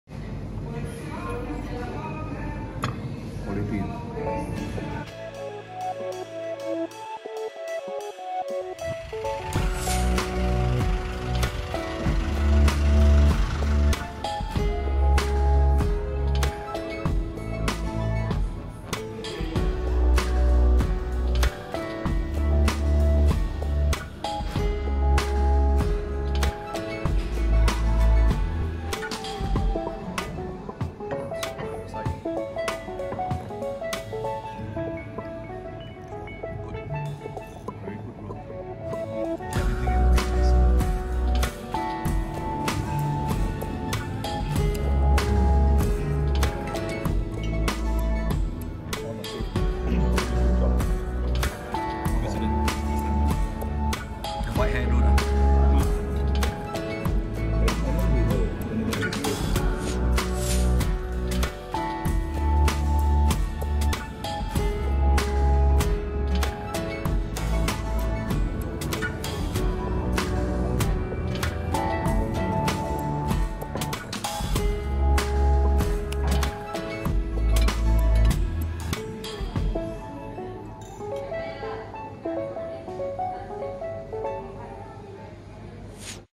Meat Paradise @ The Butcher’s Table
Tones of meat selections. Not easy to choose. If you are on meat diet. This is one place your should try! Bring 2-3 friends so can order more varieties to share!

The Butcher's Table
26, Jalan SS 2/103
47300 Petaling Jaya
Selangor